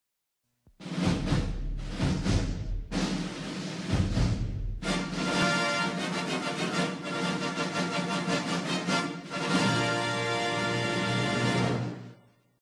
Fox Futaba Sakura Pictures Film Corporation logo (1979-1987)
Here is a Fox Futaba Sakura Pictures logo from 1976. This time with difrerent fanfare.

Released date: August 24, 1979-November 27, 1987

NOTE: Fake/Fan-made.